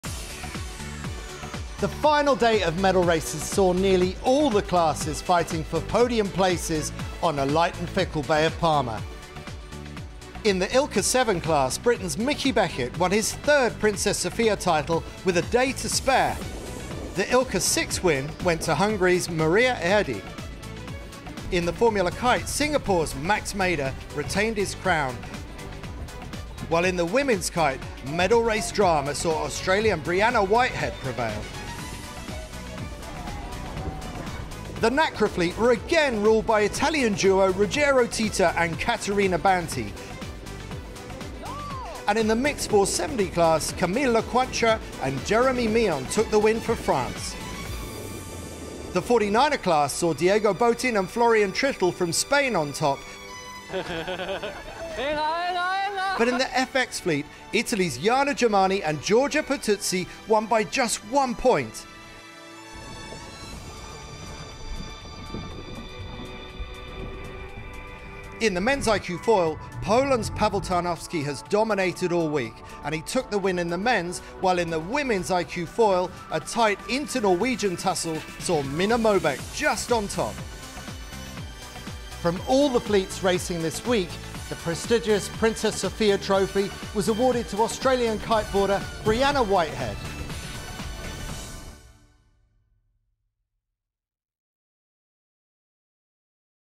Finals day | 53 Trofeo Princesa Sofía Mallorca by Iberostar
Winner of the Women’s Formula Kite event Breiana Whitehead, held her nerve under pressure as USA’s outstanding favourite Daniela Moroz false started twice, to become the only the second Australian winner ever of the overall top award as the 53 Trofeo Princesa Sofia Mallorca by Iberostar Olympic regatta concluded on the Bay of Palma today.